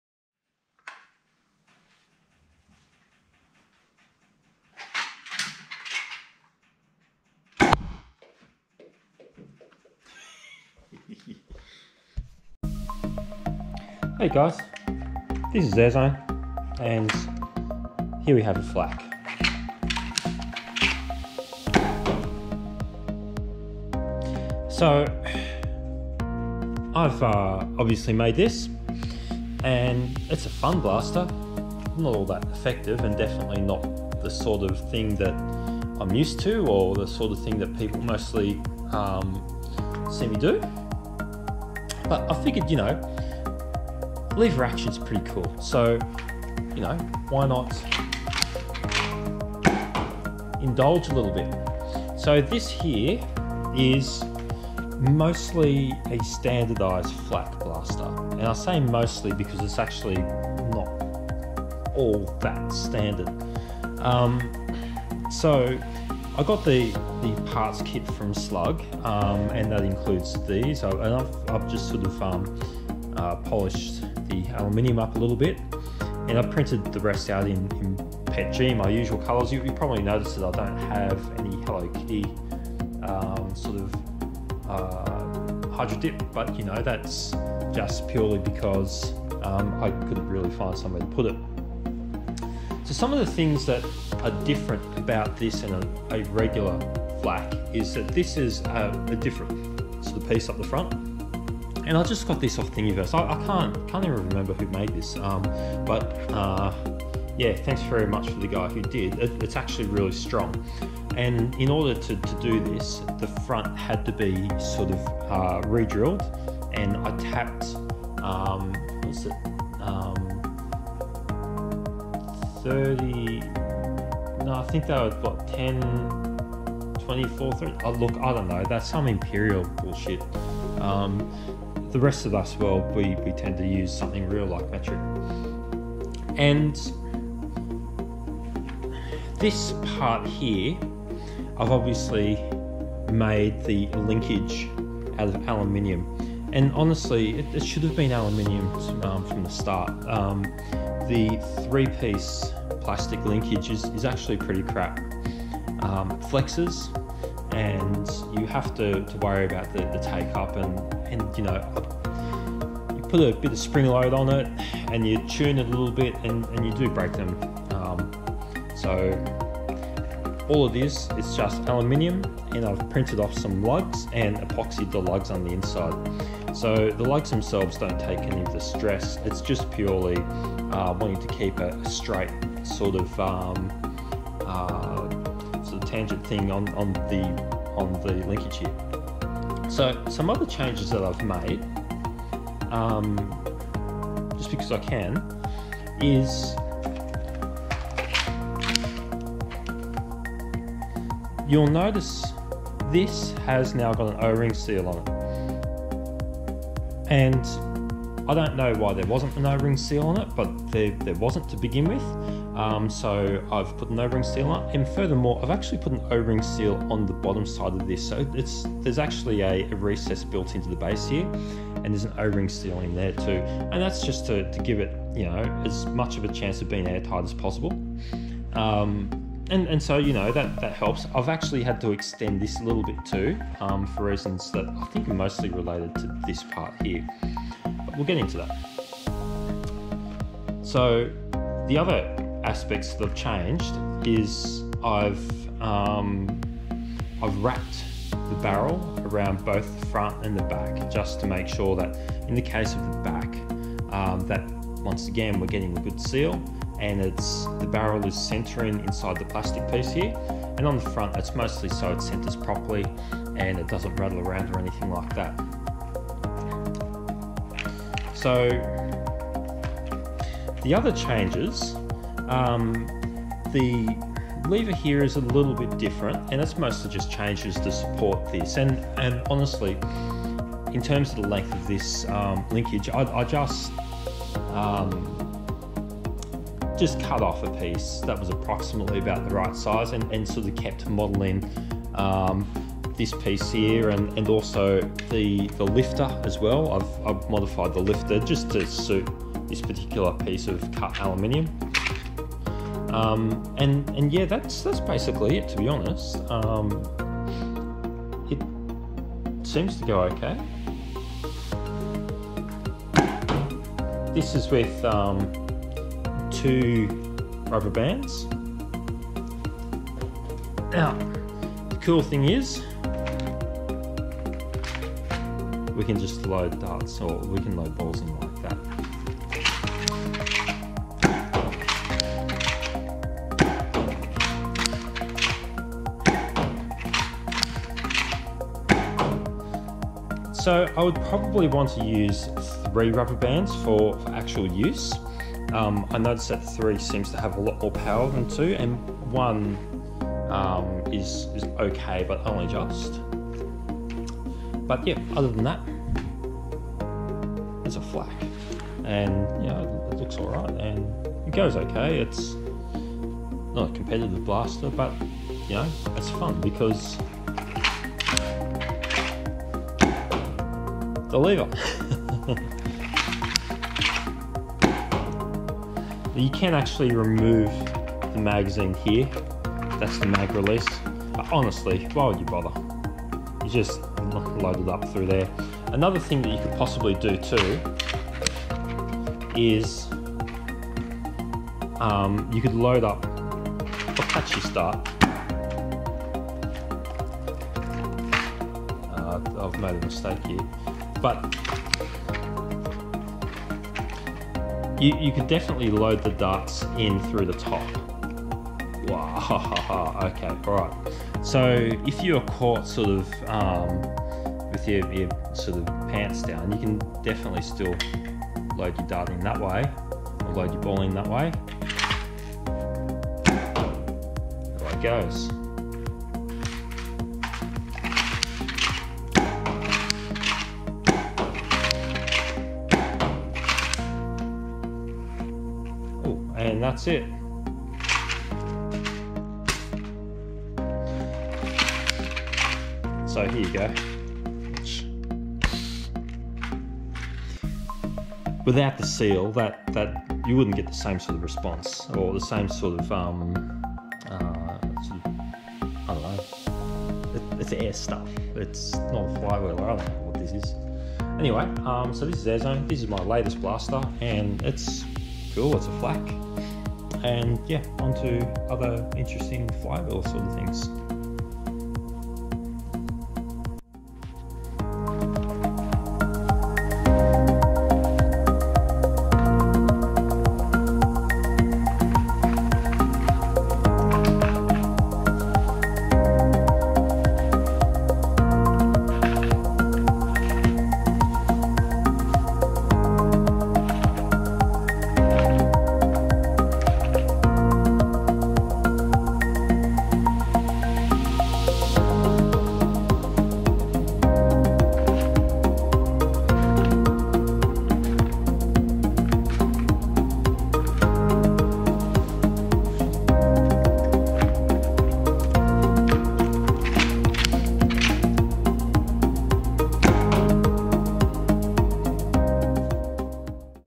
I made a FLAK for the lols
So I made a FLAK. I got a hardware kit, printed some parts, and put it all together.. But why?

Lever Action :)

I will say that the design needs a little bit of work, but in order to get a similar result, you will spend the time tweaking your models, making up a metal lever linkage, and for the love of all things holy, cleaning up aluminium parts..